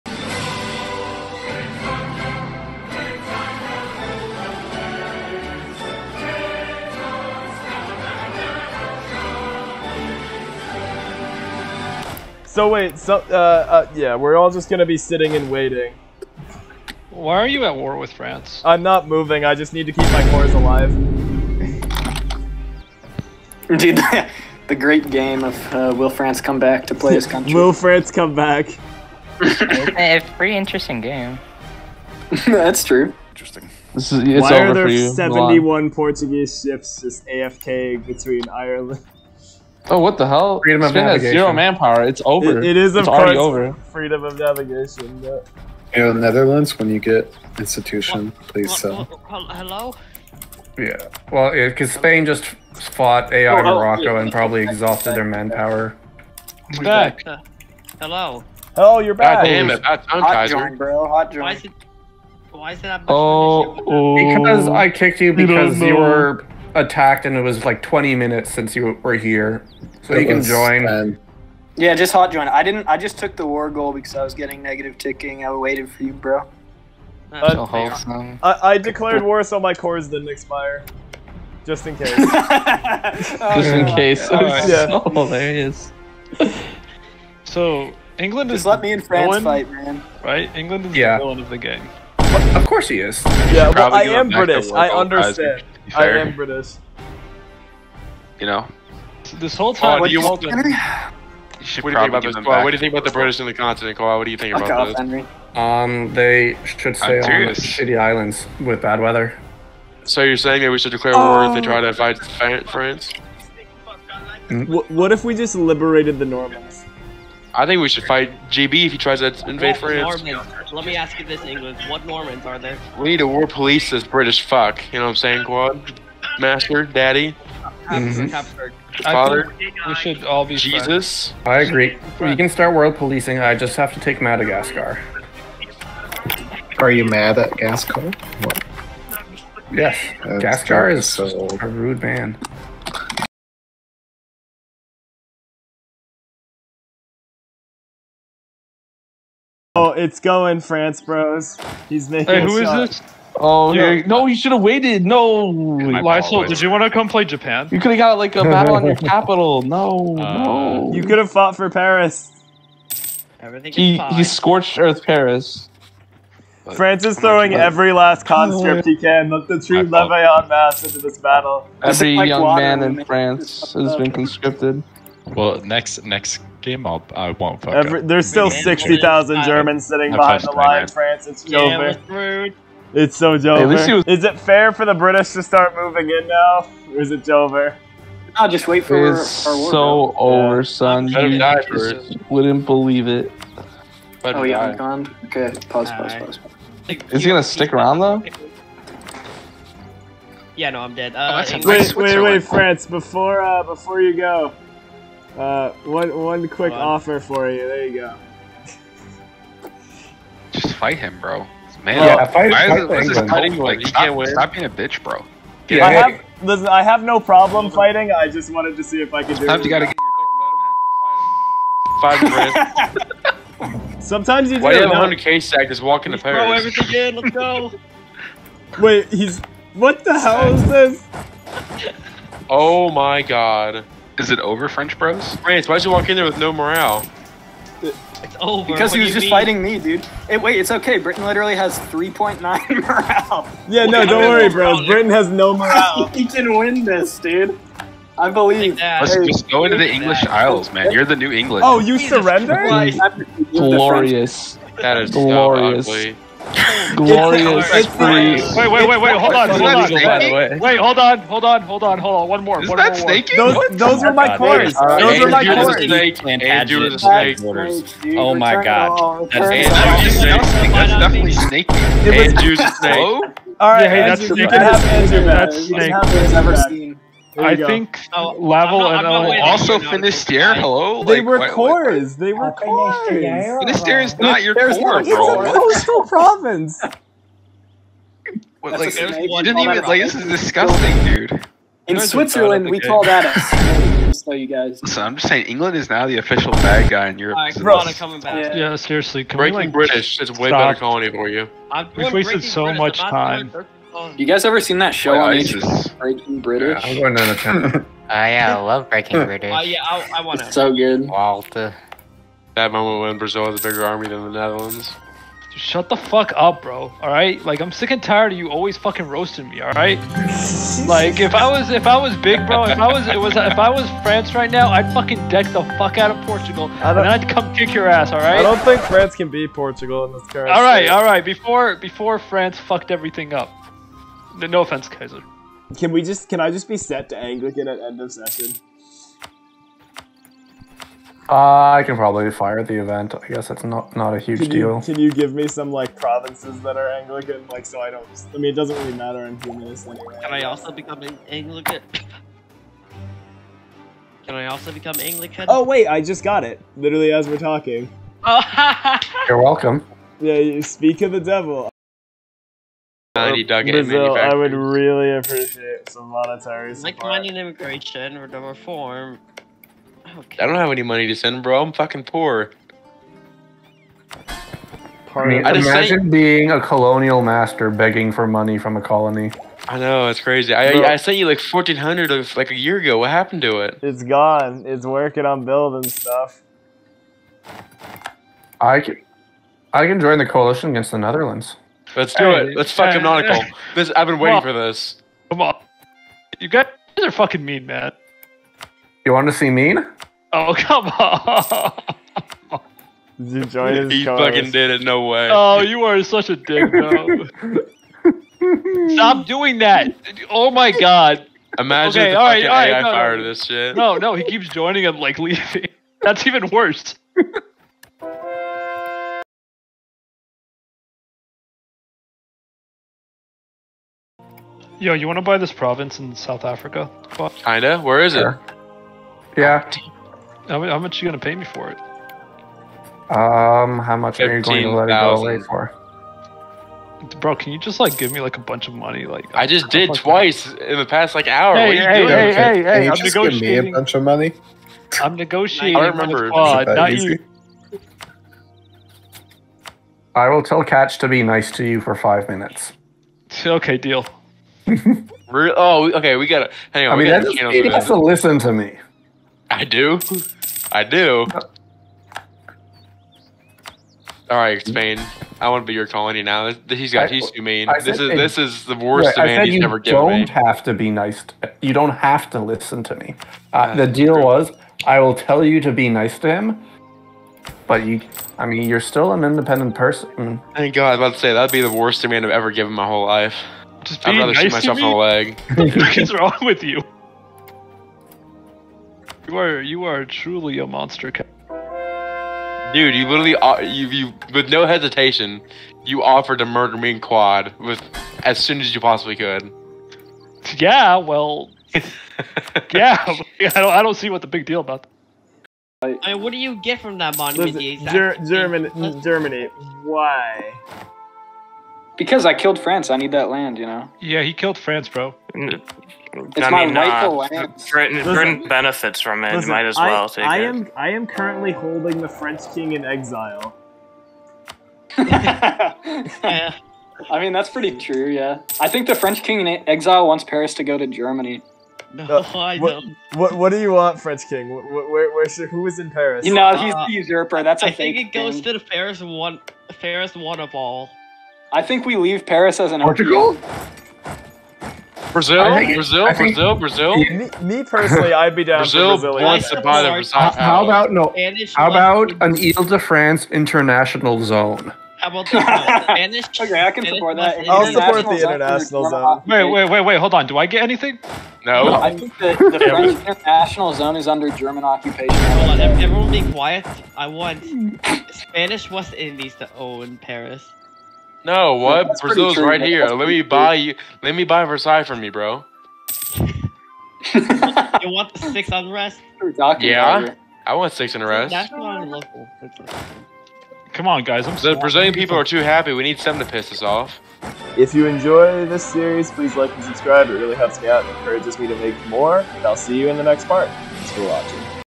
Fall of France and Dutch Bullying- England Series Episode 3
The England Series continues with the satisfying end to the Kingdom of France. We even stole their King!

If you ever want to be a part of the games I'm in here is the server that I play in!